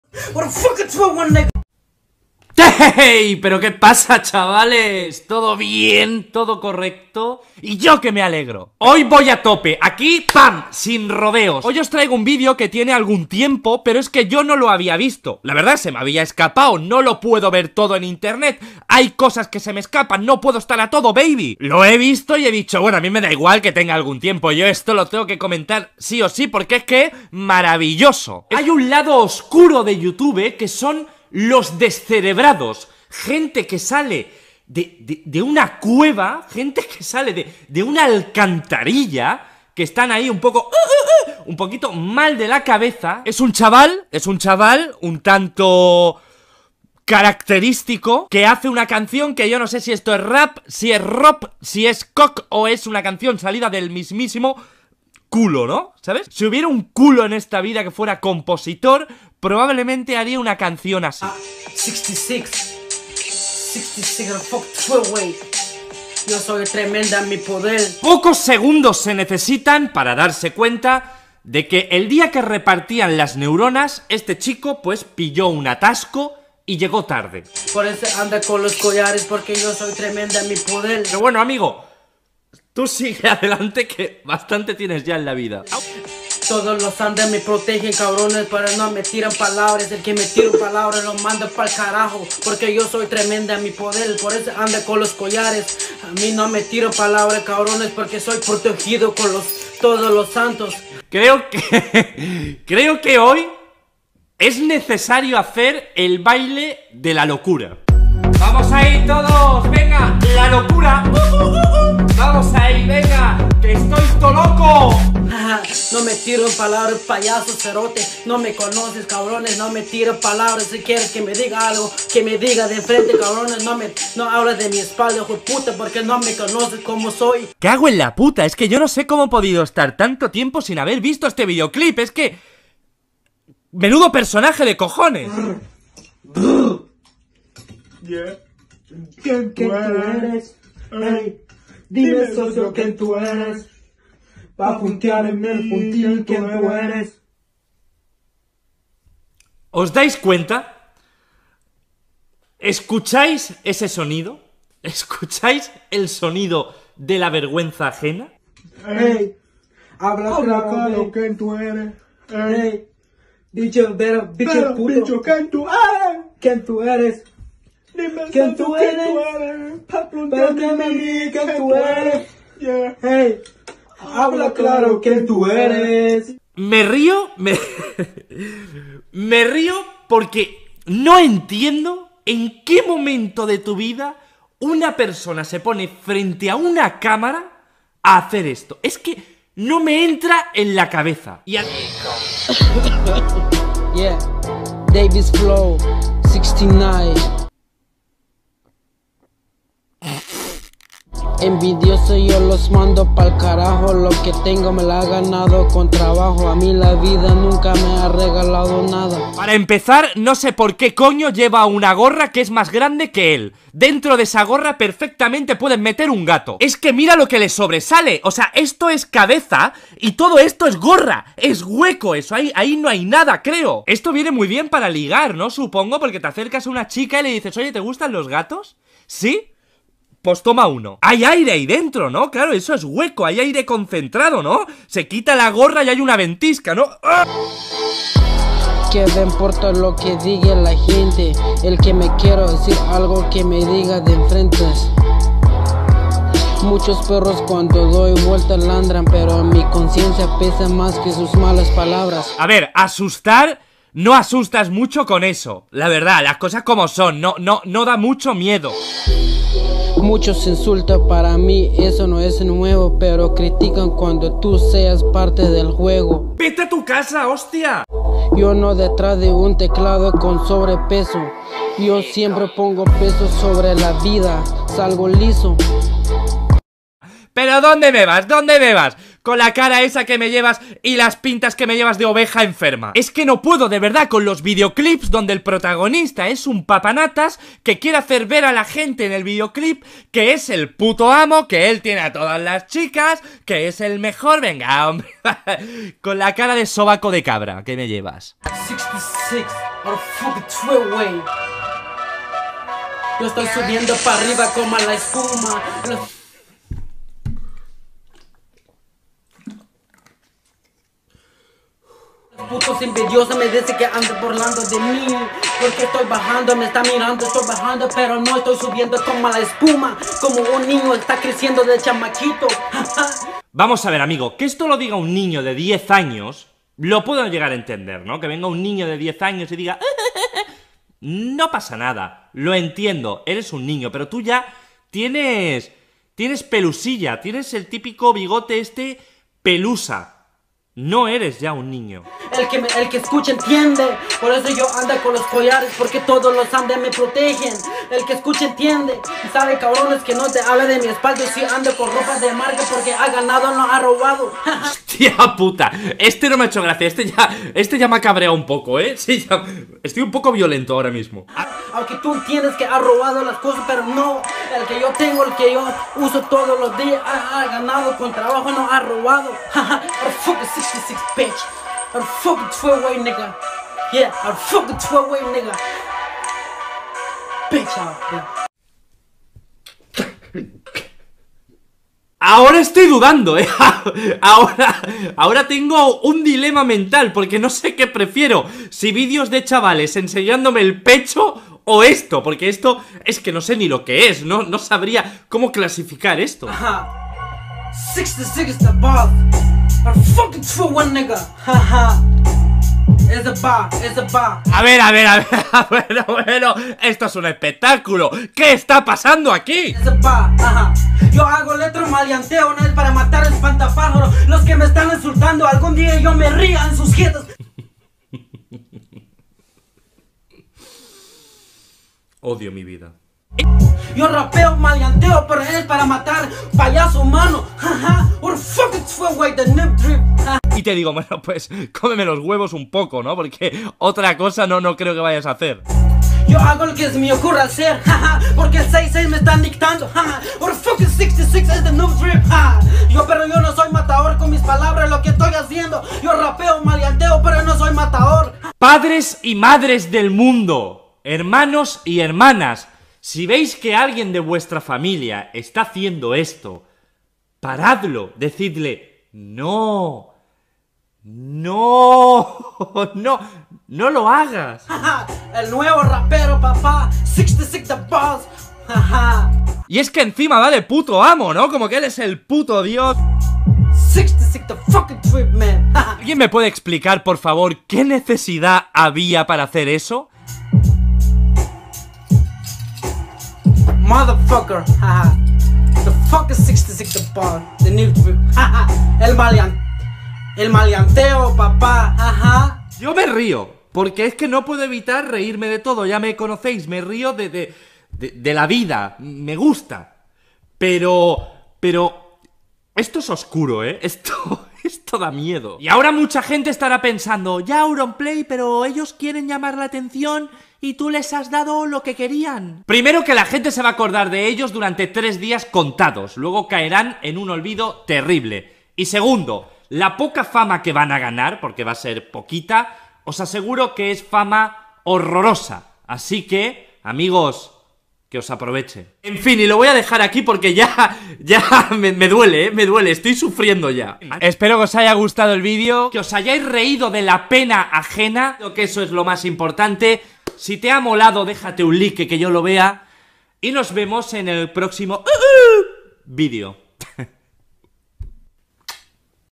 6si6six boss el primer guatemalteco en ser Rapero (parodia AEROPLAY)
Solamente es una parodia con las reacciones de aeroplay